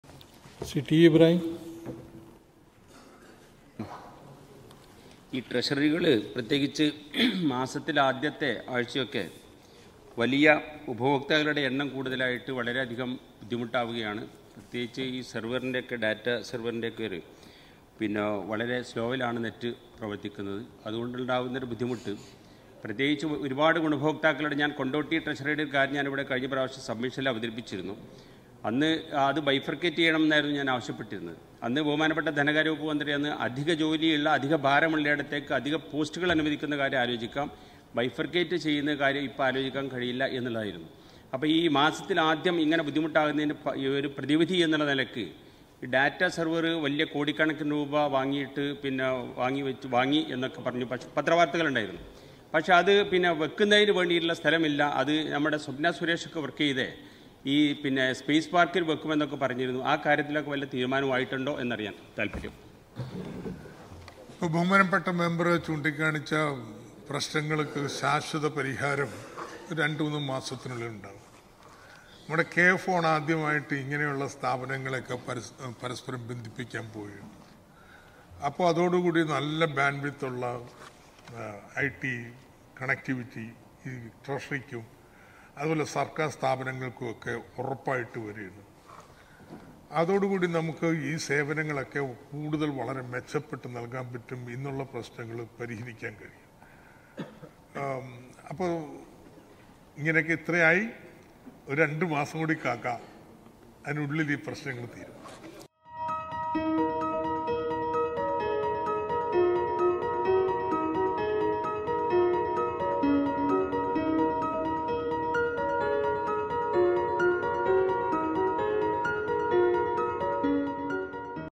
14 ാം നിയമസഭ 22 ാം സമ്മേളനം 4 ാം ദിവസം, ചോദ്യോത്തരവേള ട്രഷറികളുടെ പ്രവർത്തനം മെച്ചപ്പെടുത്താൻ നടപടി
15th KLA , 5th session 14 ാം നിയമസഭ 22 ാം സമ്മേളനം 4 ാം ദിവസം, ചോദ്യോത്തരവേള ട്രഷറികളുടെ പ്രവർത്തനം മെച്ചപ്പെടുത്താൻ നടപടി